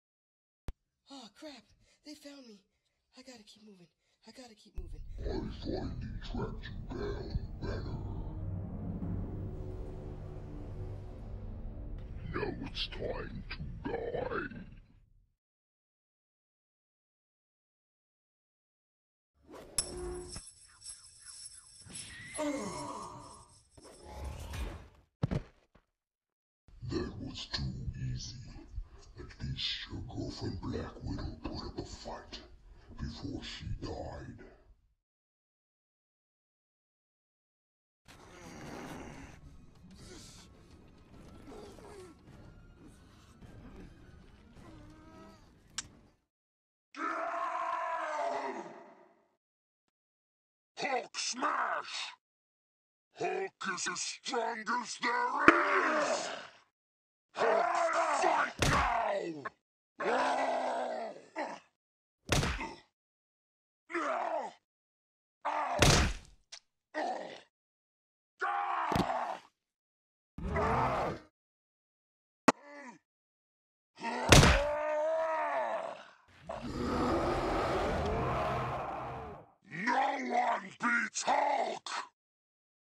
Hulk vs Cull Obsidian Stop Motion
Bruce Banner finally becomes the Hulk once again to battle Cull Obsidian of the Black Order.